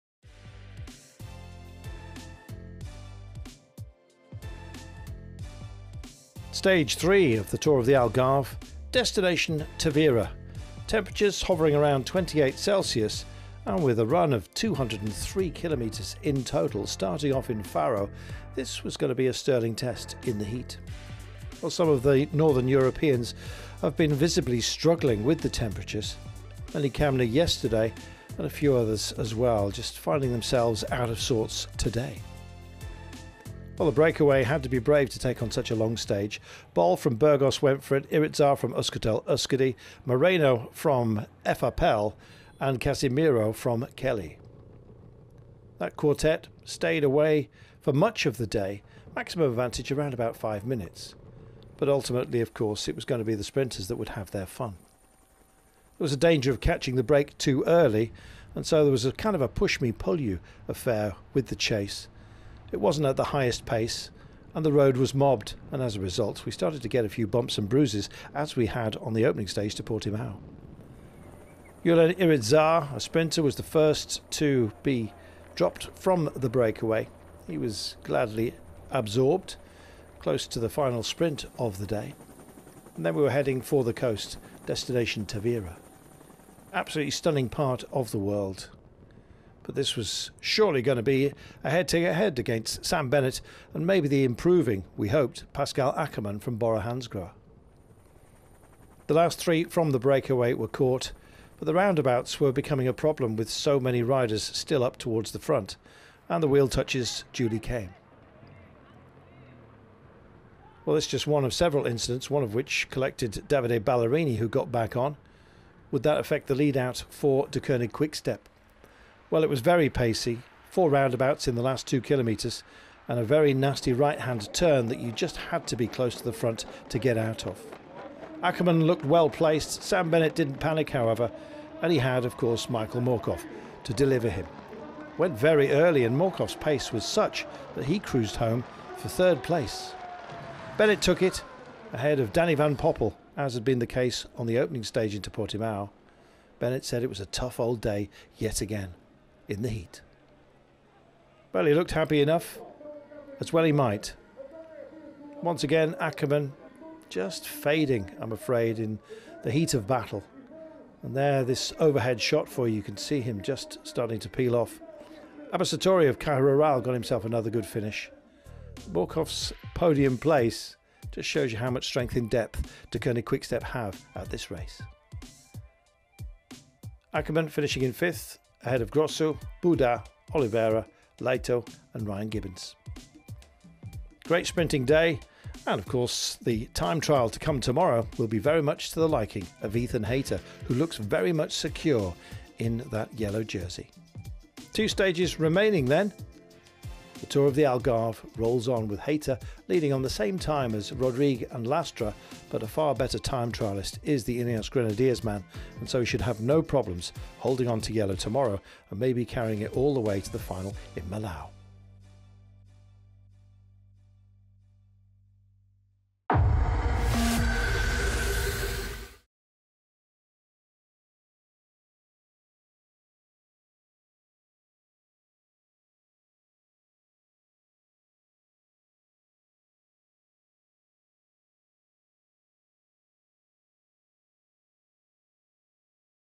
Volta ao Algarve 2021 - Stage 3 Highlights | Cycling | Eurosport
Watch the highlights from stage 3 of the Volta ao Algarve 2021.